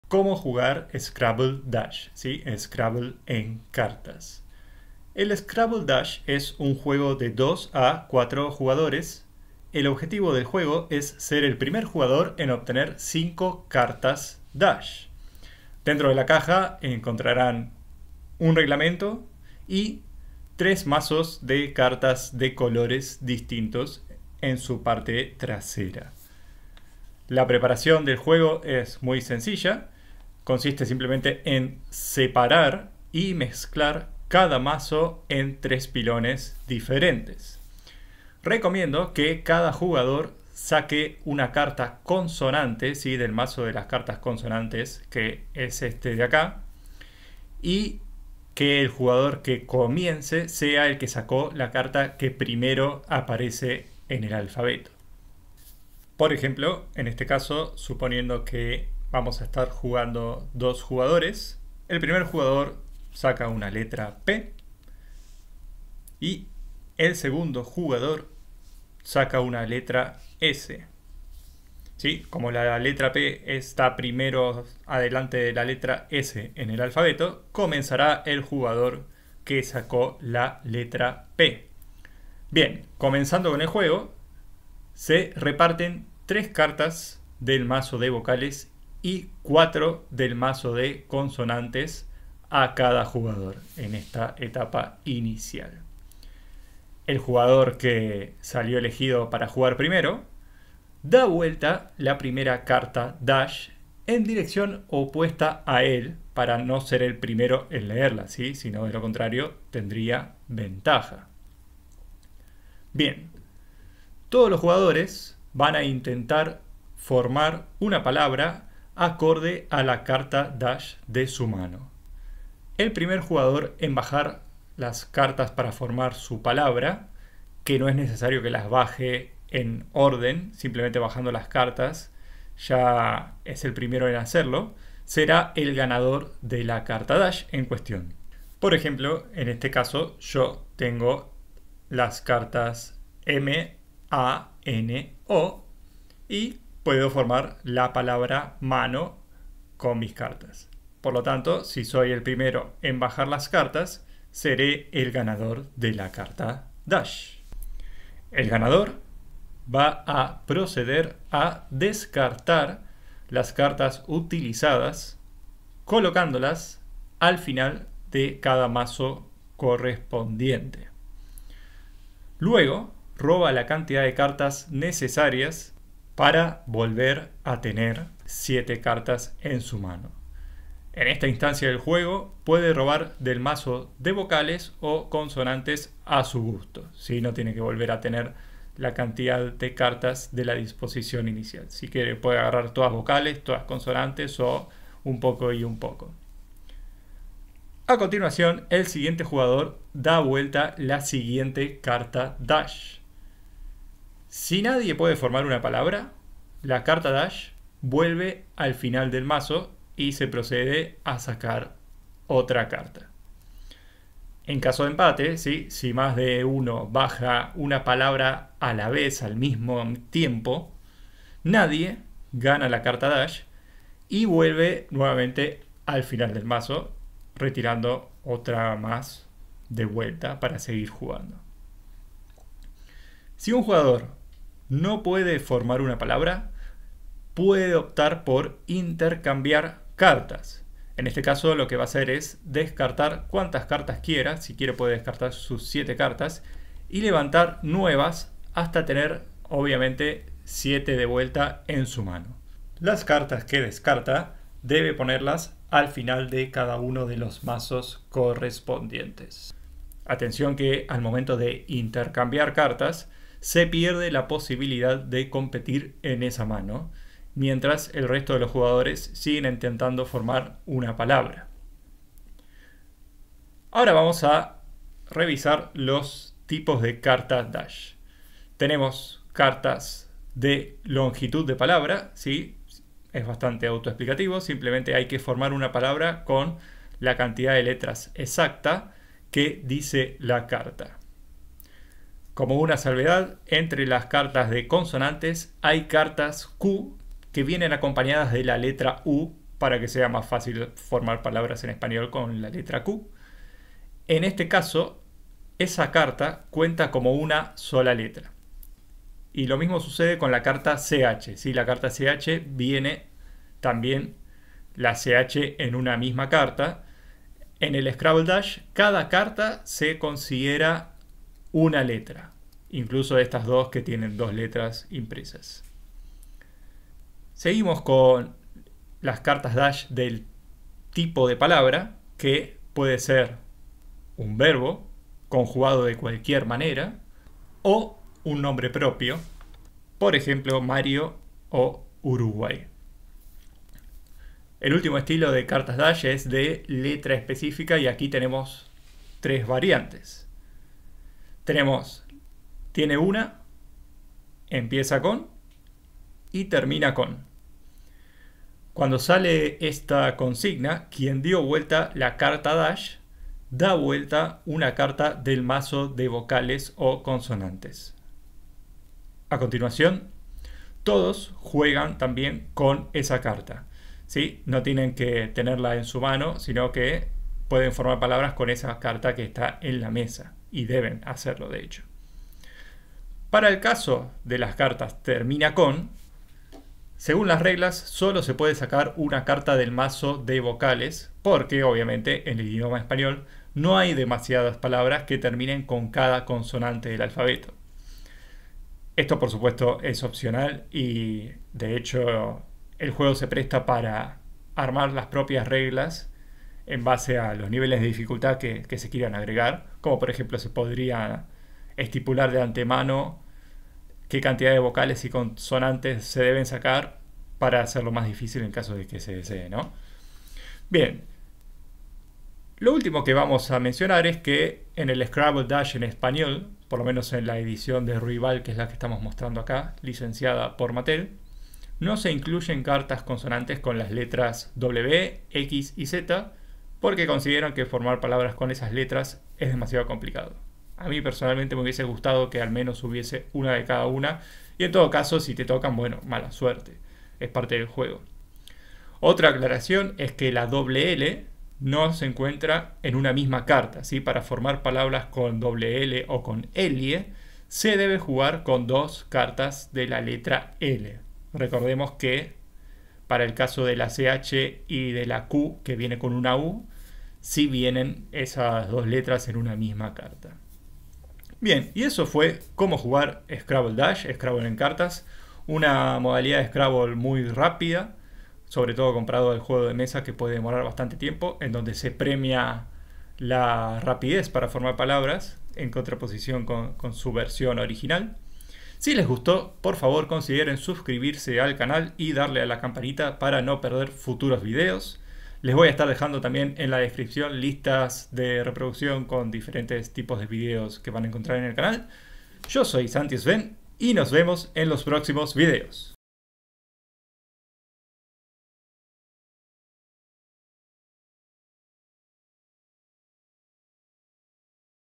Cómo Jugar SCRABBLE DASH 🃏 🅰️ 🅱️
Scrabble Dash: ¿Cómo se juega? Reglas del juego.

0:00 Intro
0:09 Objetivo
0:15 Contenido
0:27 Preparación
1:22 Comenzando a jugar
3:53 Intercambiar cartas
4:33 Tipos de cartas Dash
7:02 Aclaraciones

¡SEGUIME! 👍